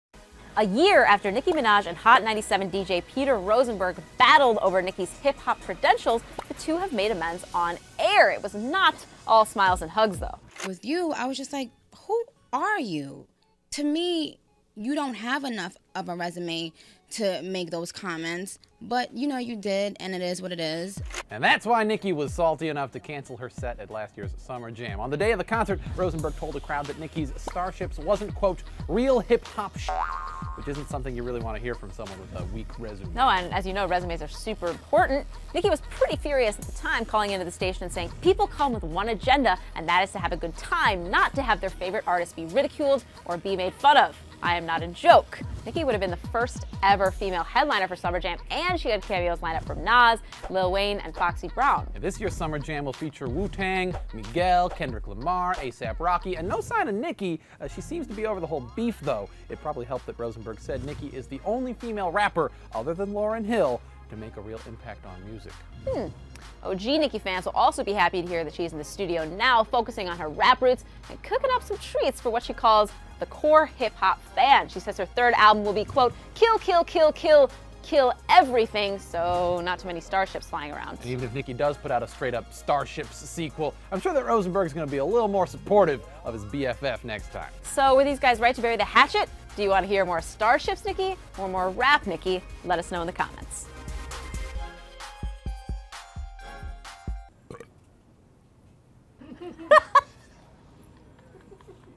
Nicki Minaj & Hot 97's Peter Rosenberg Bury Summer Jam Beef
Nicki Minaj and Hot 97's Peter Rosenberg feuded at Summer Jam 2012; a year later, they made up and spoke about Nicki's new music.